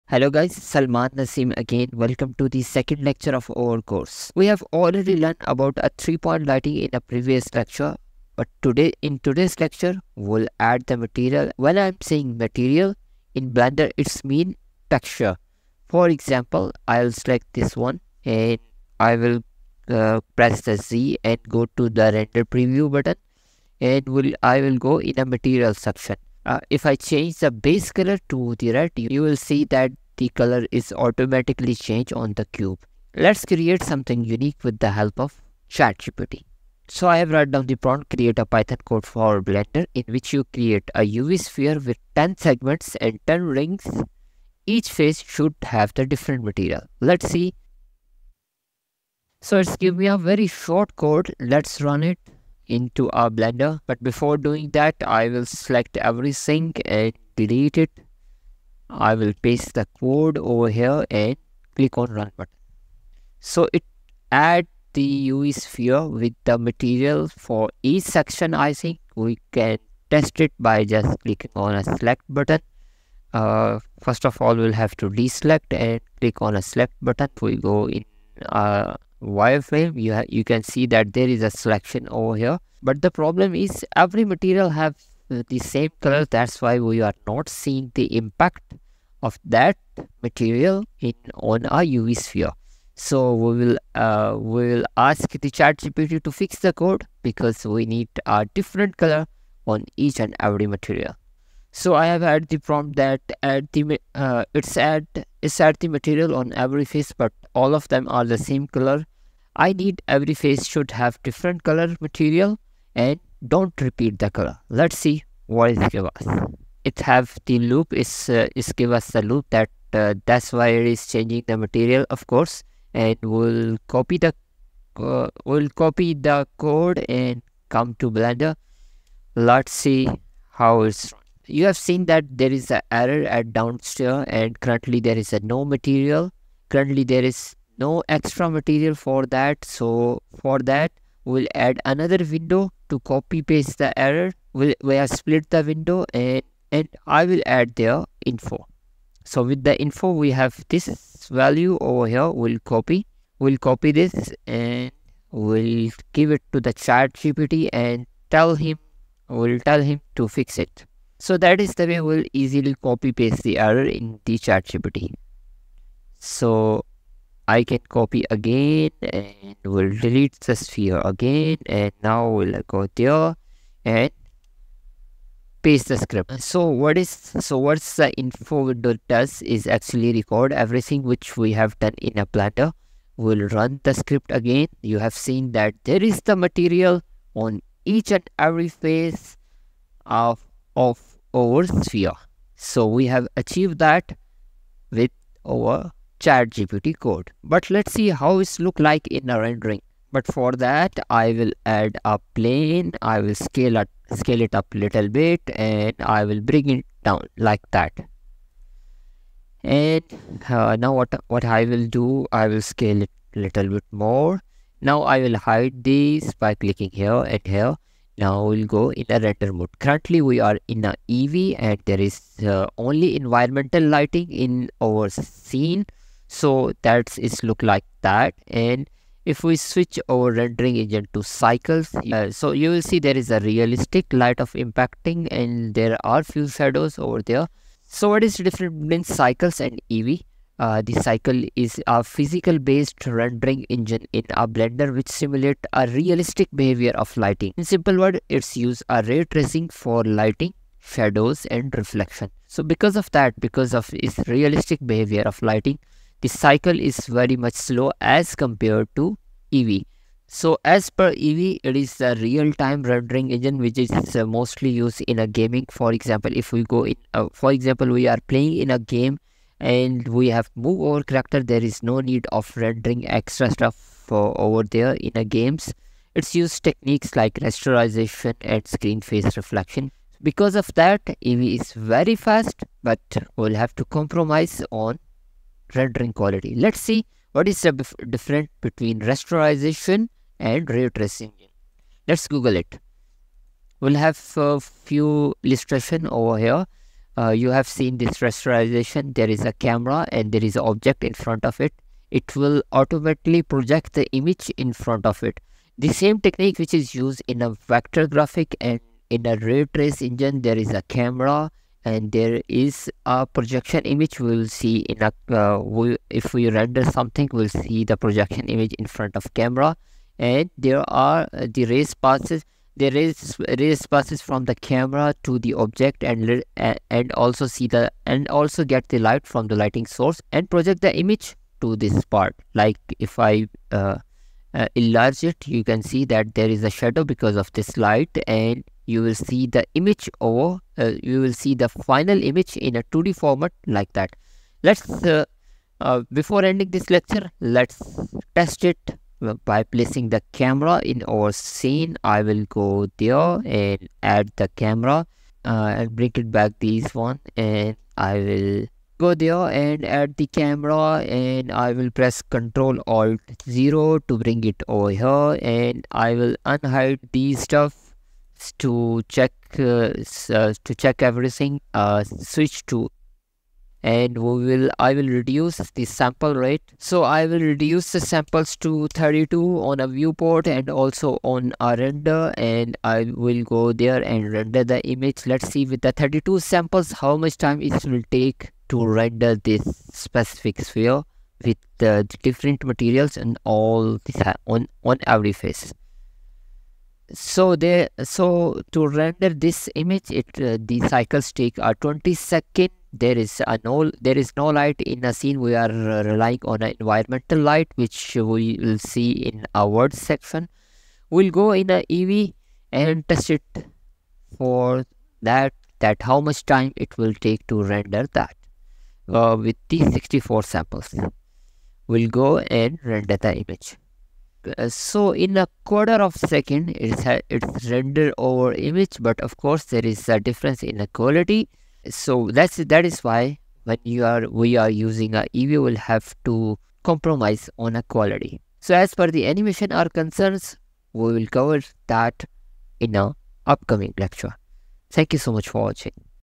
ChatGPT + Blender part 2 | Material Assignment with help of Python | Being Animator Tutorial
Welcome to the second installment of our Blender + ChatGPT series by Being Animator! In this Tutorial, unravel the techniques of assigning unique materials to each face of a sphere, enhancing your 3D art. We will walk you through manual rendering processes in both Eevee & Cycles. Whether you're refining your skills or exploring new horizons, this tutorial is your gateway to mastering Blender with the insights from ChatGPT!

00:00 Material For Beginners
00:39 Generating Python Code
02:10 Fix Python error in blender
03:05 Different material on Everyface
04:41 Ray tracing vs rasterization
05:55 EEVEE vs Cycle Render time